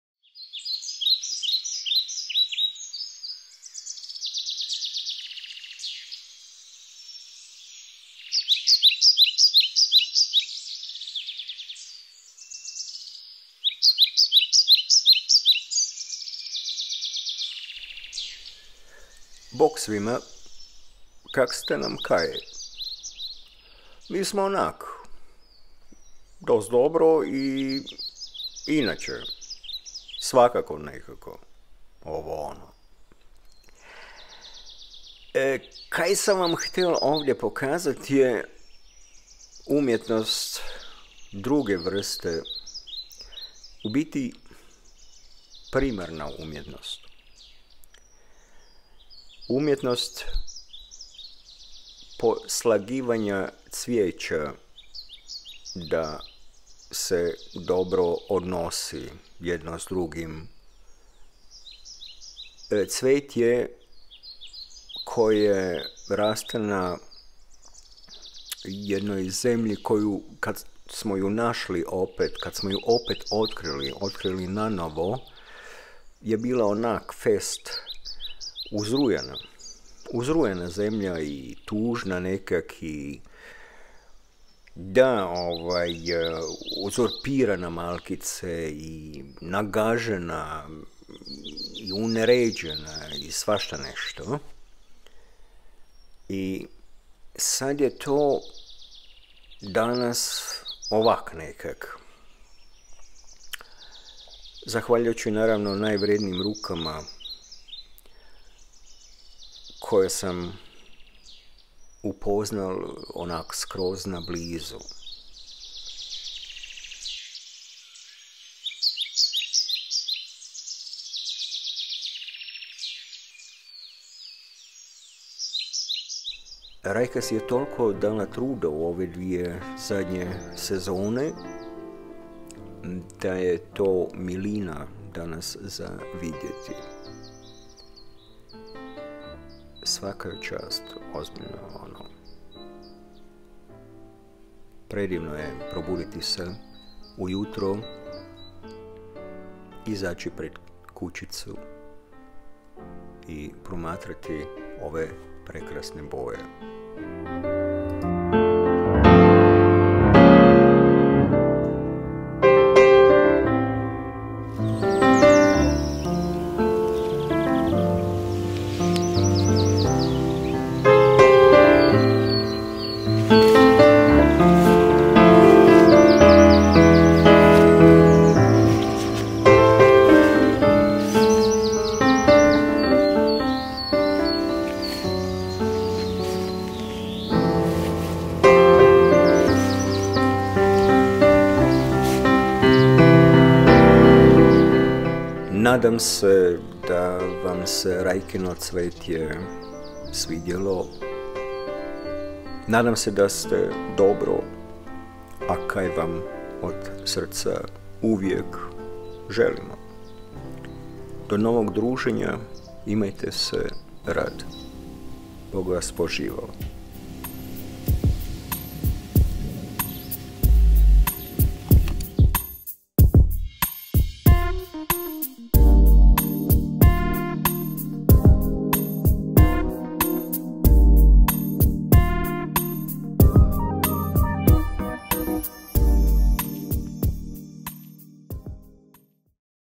Rajkin cvijetnjak u jednom malom, jako lijepom hrvatskom, moslavačkom selu. Pogodite kojem!
Mali znak pažnje, ljubavi i zahvalnosti prema cvetju i skoro pa najvrijednijim rukama koje su i mene (a ne samo cvetje u filmu) milovale!